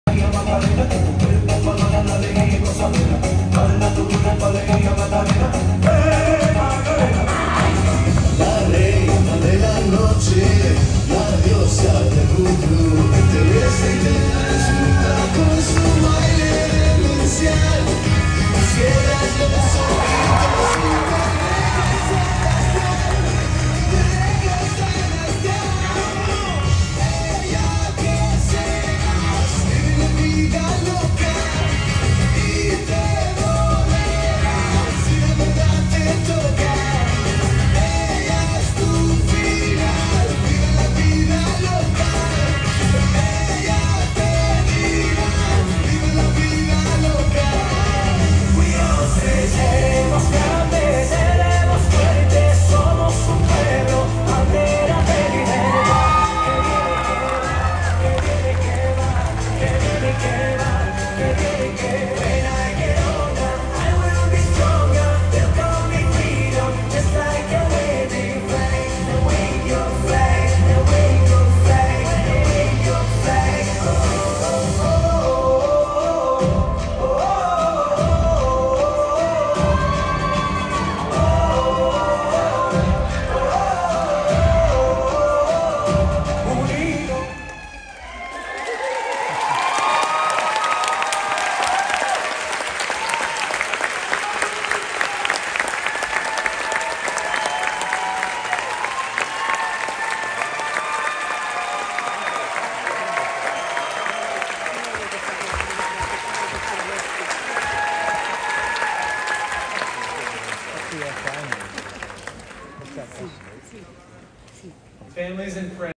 4J School District Graduation Day - dance 2022
Graduation program of IHS  , 4j school District . / 06/10/2022 @ Hult Center, Eugene